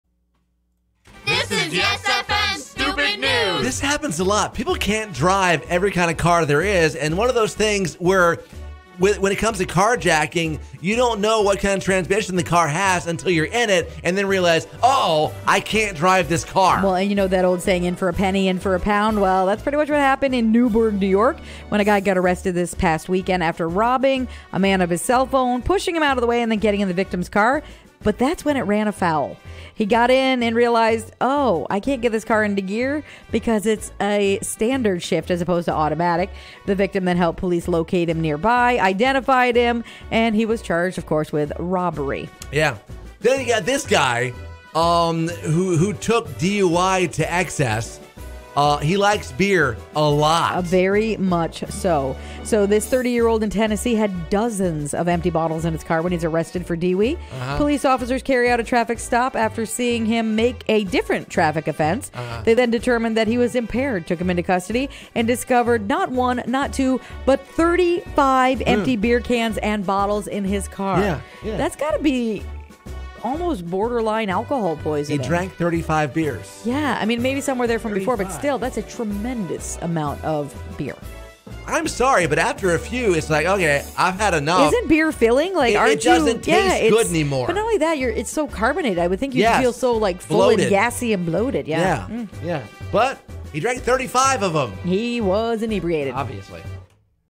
Cant drive stolen car & 35 beers later
A trespasser in Newburgh, New York, was arrested last Saturday after he robbed a man and A 30-year-old Tennessee man had dozens of empty bottles in his car when he was arrested for DUI arrest.